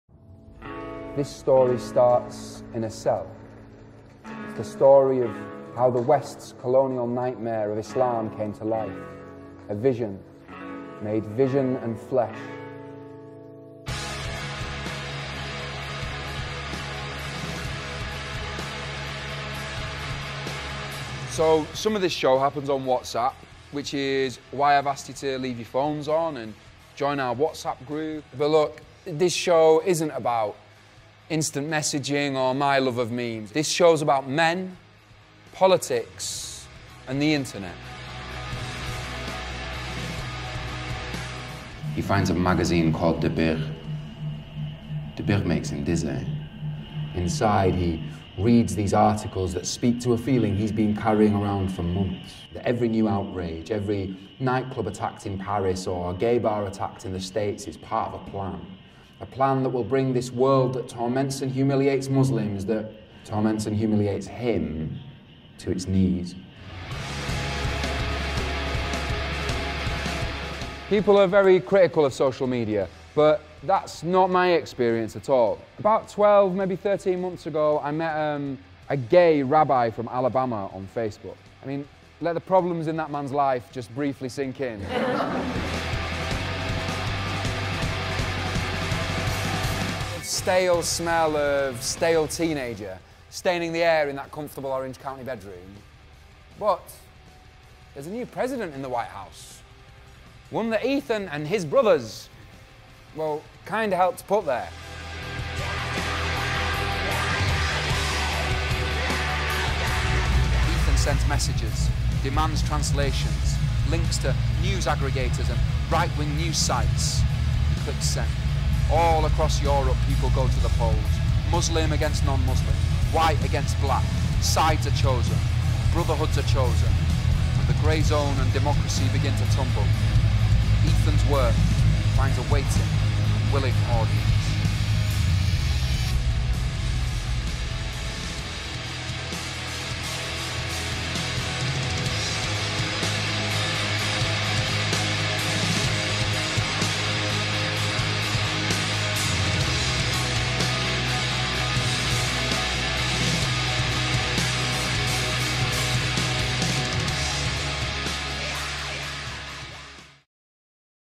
The Believers Are But Brothers
Written and performed by Javaad Alipoor
Co-directed by Javaad Alipoor and Kirsty Housley

We live in a time where old orders are collapsing: from the postcolonial nation states of the Middle East, to the EU and the American election. Through it all, tech savvy and extremist groups rip through twentieth century political certainties.

Amidst this, a generation of young men find themselves burning with resentment; without the money, power and sex they think they deserve. This crisis of masculinity leads them into an online world of fantasy, violence and reality.

Writer and theatre maker Javaad Alipoor spent time in this digital realm, exploring the blurry and complex world of extremists, spies, journalists and fantasists. This bold show weaves together their stories.